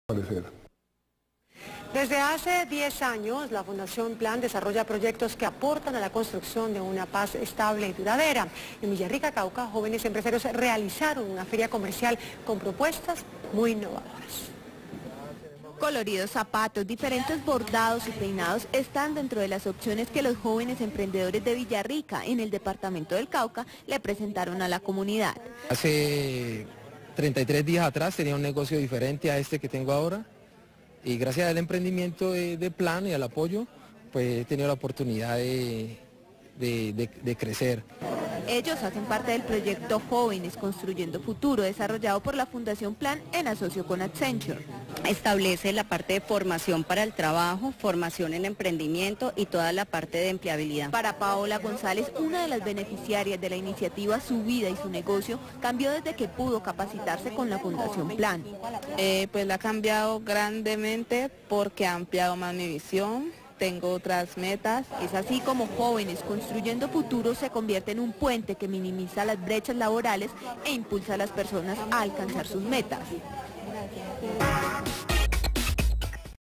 "Jóvenes Construyendo Futuro"
PLAN lleva más de 10 años implementando proyectos que aportan a la construcción de paz y especialmente acerca de la vital importancia de la participación de las niñas, niños, adolescentes y jóvenes en la construcción de una paz territorial estable y duradera. Todos los proyectos se enmarcan en esta iniciativa, este es el caso de Jóvenes Construyendo Futuro desarrollado por PLAN en asocio con Accenture.